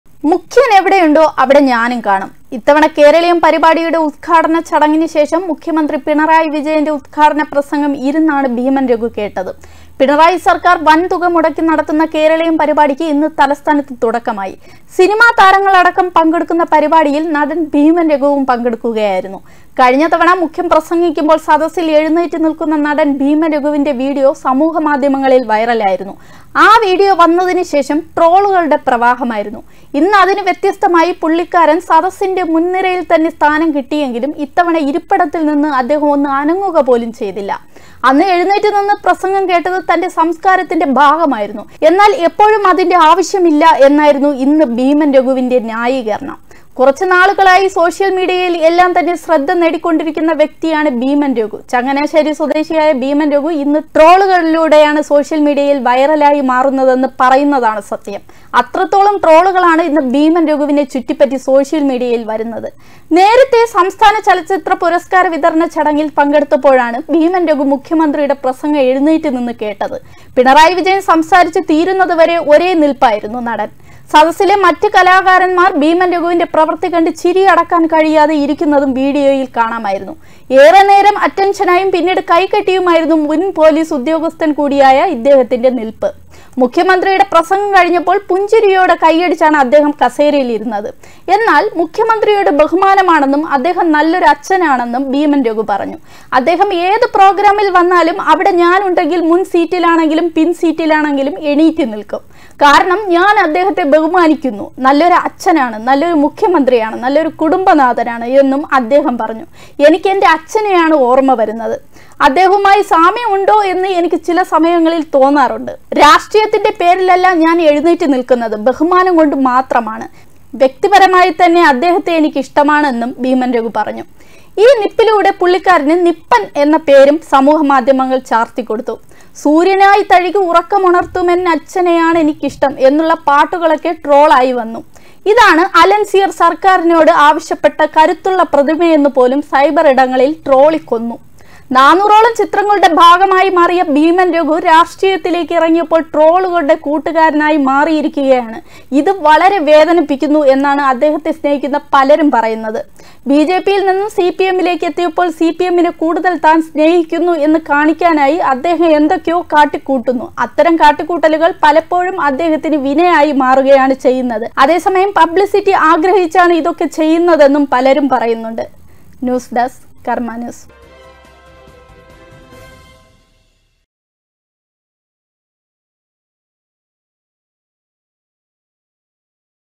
മുഖ്യന്റെ പ്രസംഗം ഇരുന്ന് കേട്ട് ഭീമൻ രഘു
കേരളീയത്തിൽ പങ്കെടുക്കാൻ എത്തിയ ഭീമൻ ,മുഖ്യന്റെ പ്രസംഗം ഇരുന്ന് കേട്ടു